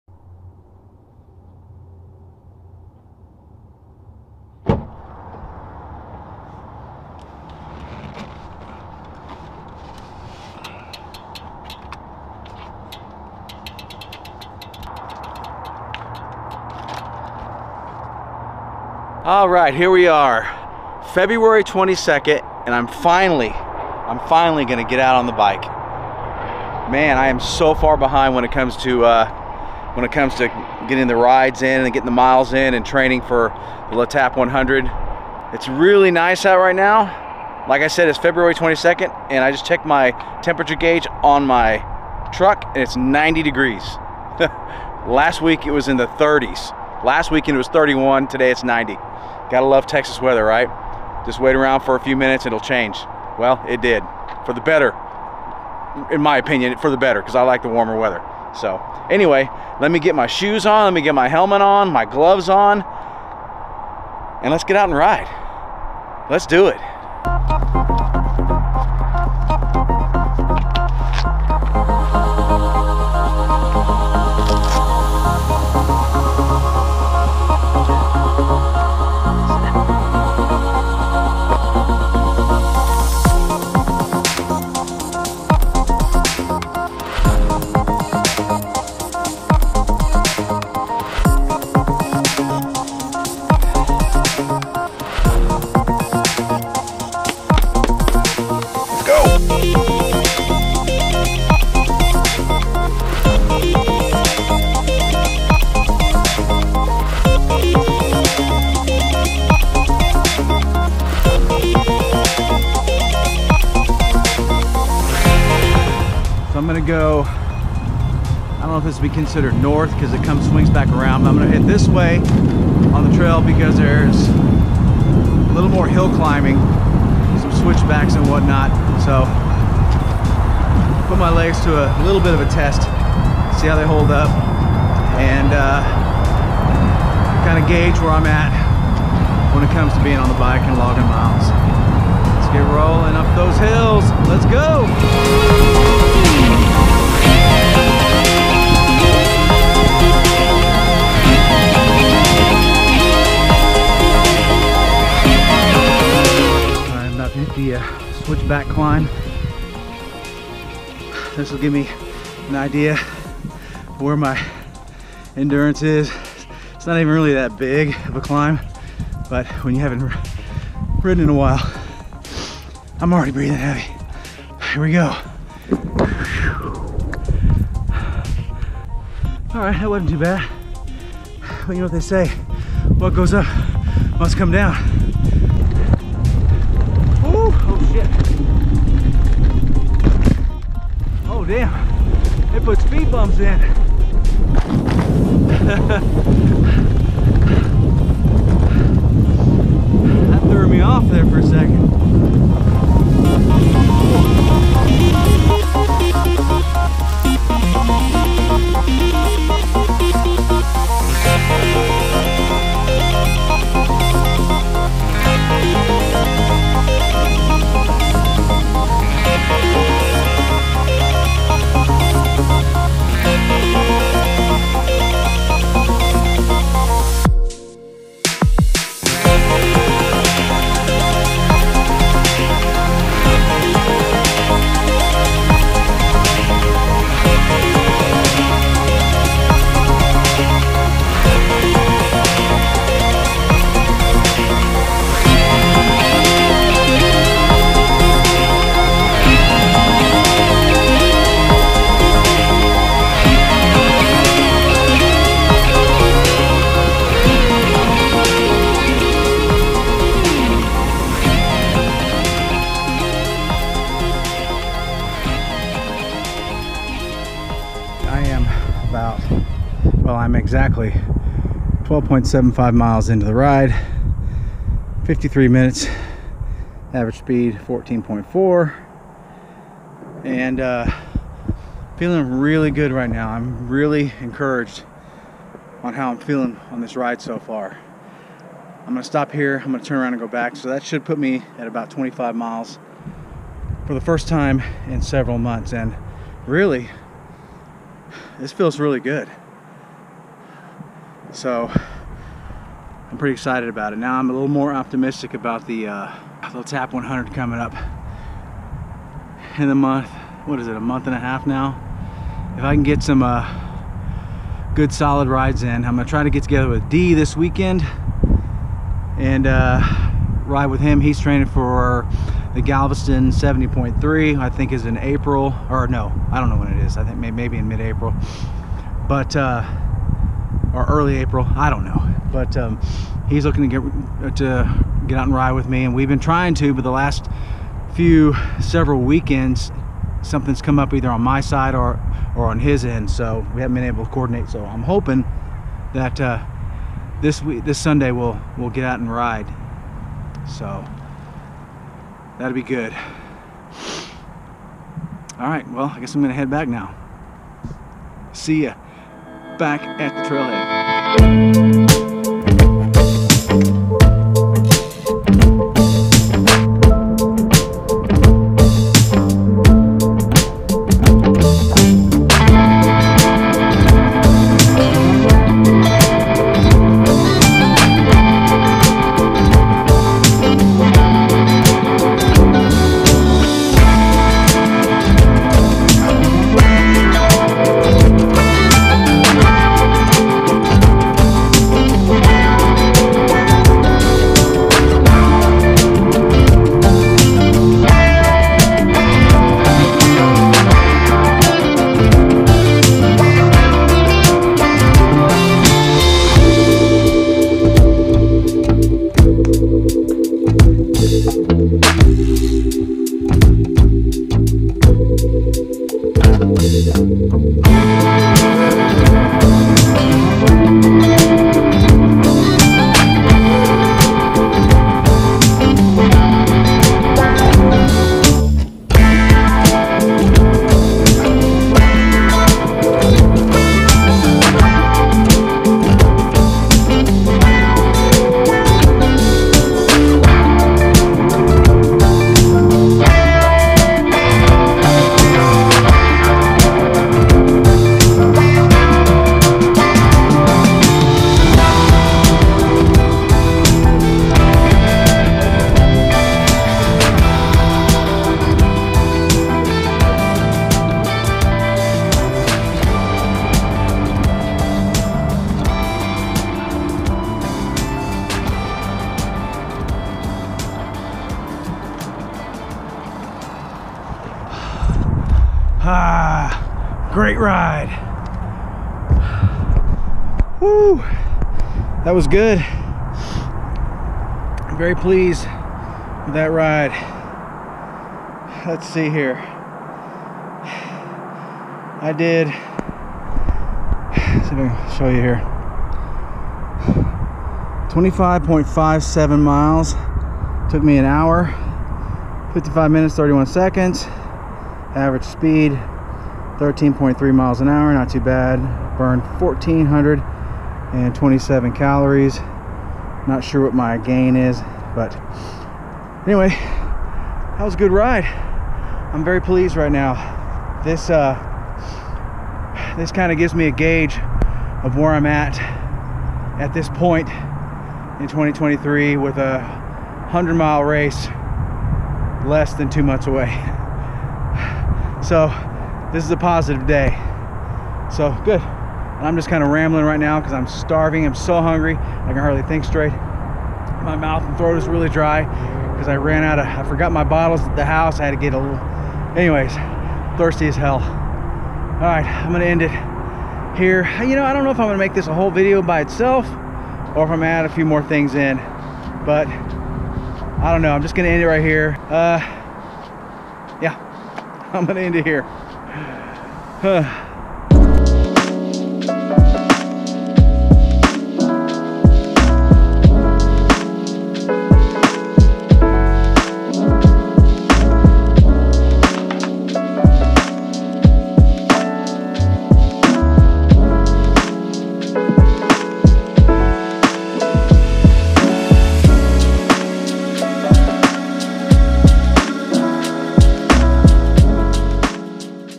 Getting a late start!/First ride of 2023/LeEtape 100 a month and a half out!/Will I be ready?!
The LeEtape 100 is about a month and a half away and this is my first ride of the year! Definitely getting a late start for sure! But better late than never! If I can get some solid tides in I should be able to knock out the 100 miles! I guess we'll have to wait and see.